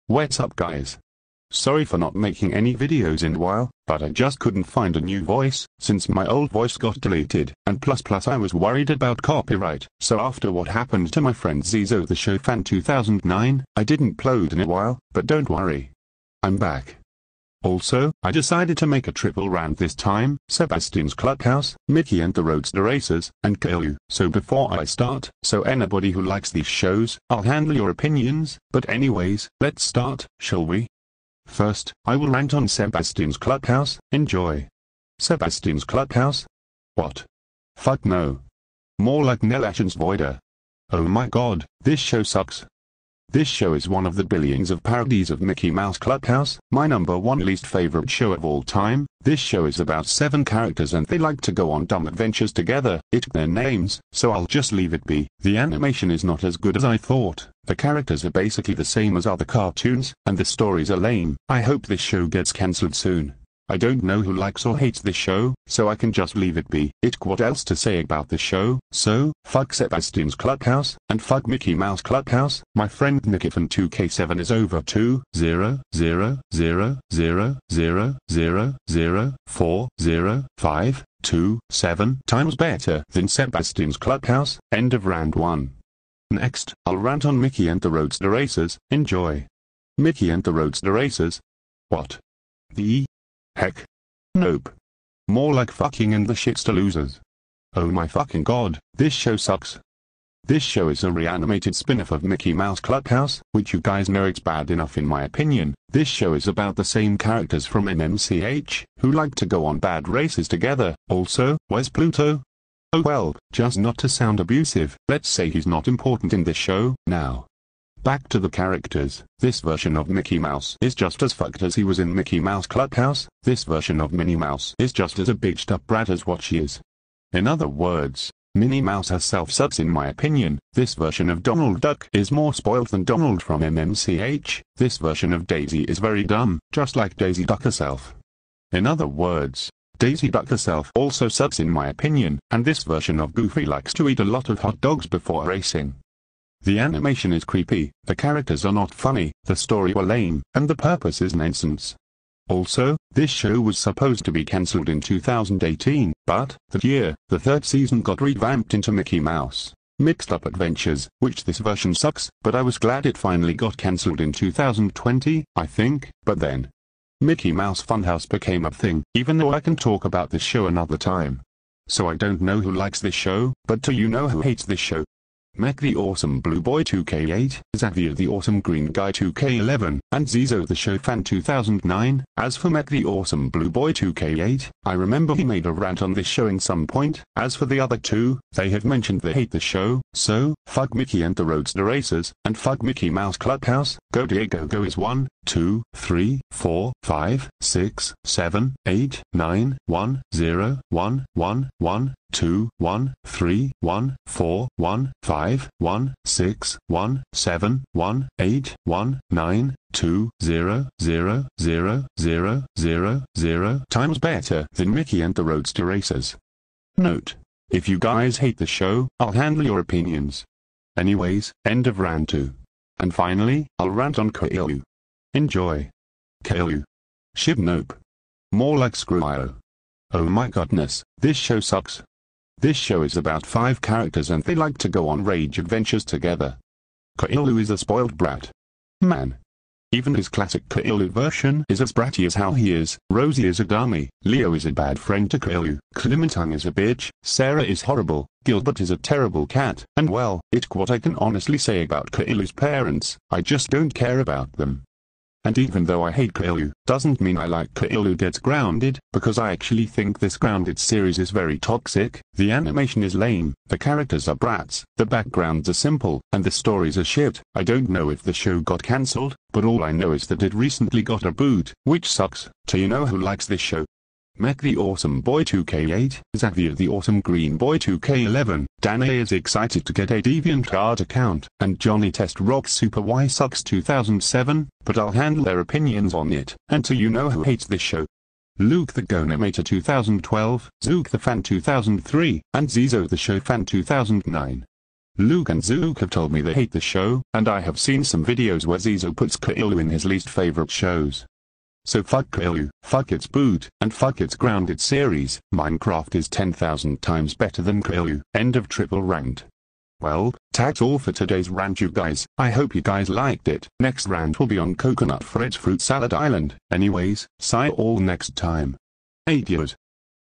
Joseph Triple Rants #10: Sebastian's Clubhouse, Mickey And The Roadster Racers, and Caillou
This is my official return to video making! I wanted to return in a special way, by making a triple rant, 1 of them was suggested, the other 2 were just to fold up the rant!

NOTE: if you guys like any of these shows i ranted on, i'll handle your opinions!